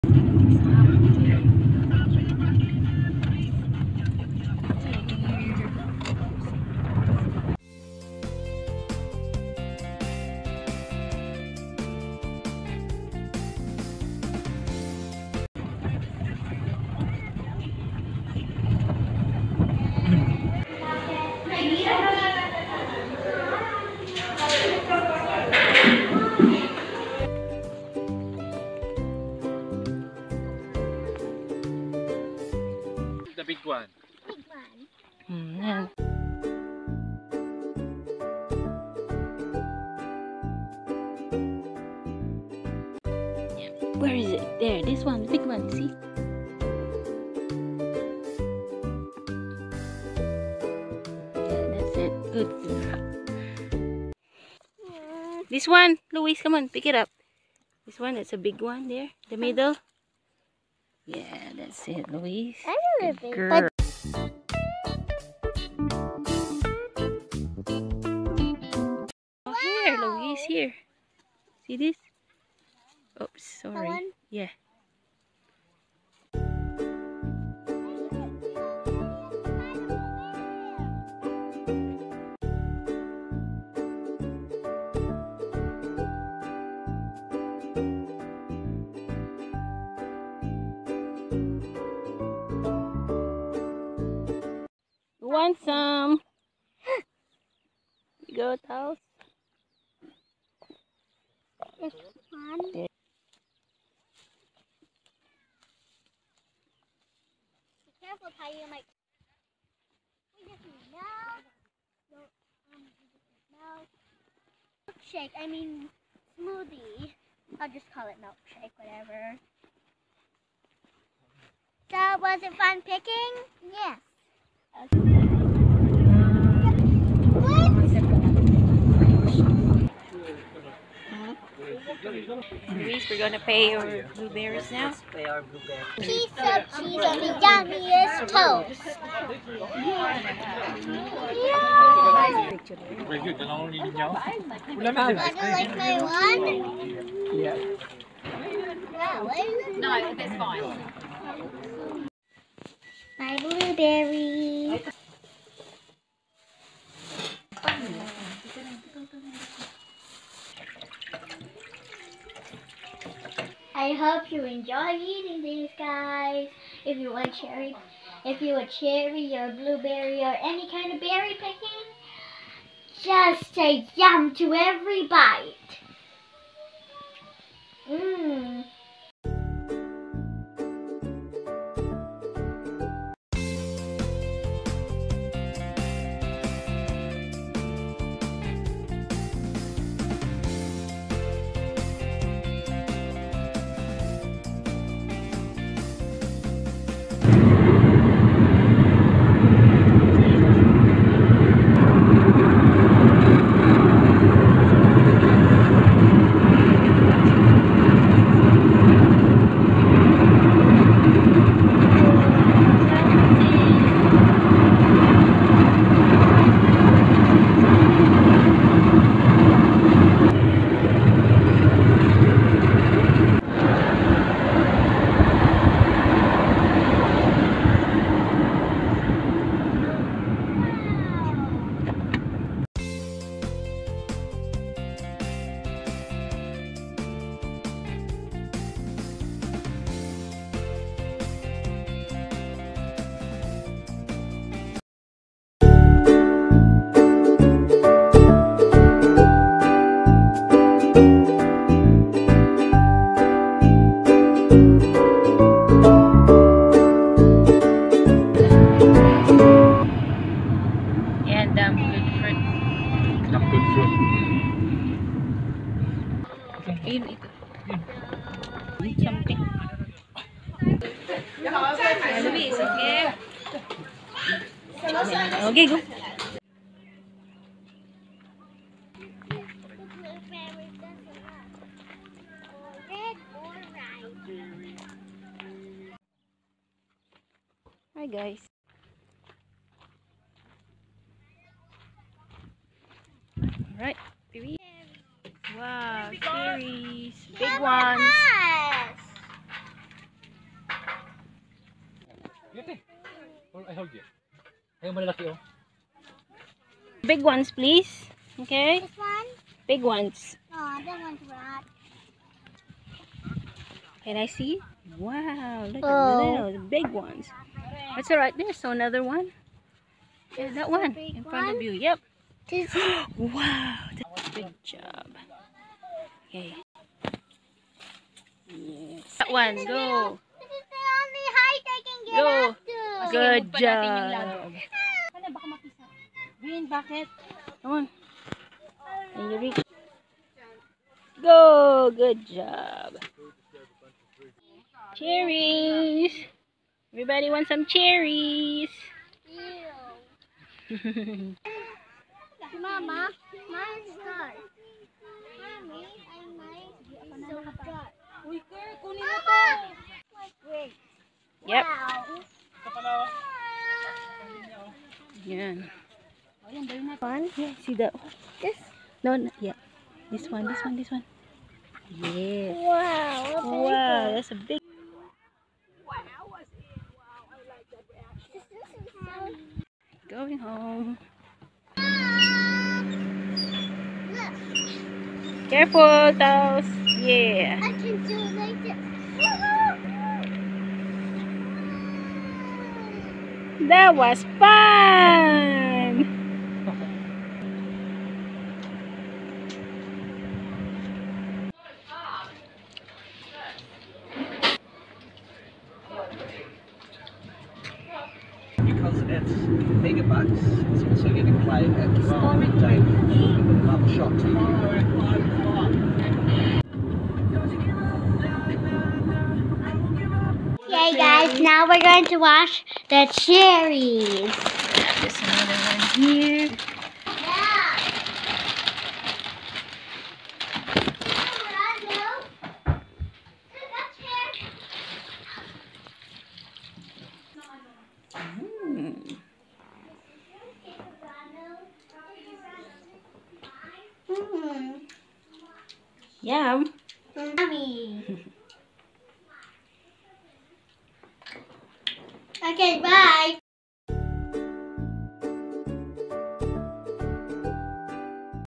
Blueberry and Cherry picking | Otautau | Cromwell | Louise Lubos
We enjoyed blueberry picking in Blueberry Country Southland at Otautau and Cherry picking in Dam Good Fruit at Cromwell. It was worth the drive on our Day 1 and Day 2 travel to the berry and cherry orchards. Had a fun experience picking blueberry and cherry with my family. The season of berries are usually from December to January-midFebruary. So if you want to pick some yummy, fresh berries and cherries, visit on those months! Watch and enjoy this video during our trip to both orchards!!!  :) YouTube Channel : Louise Lubos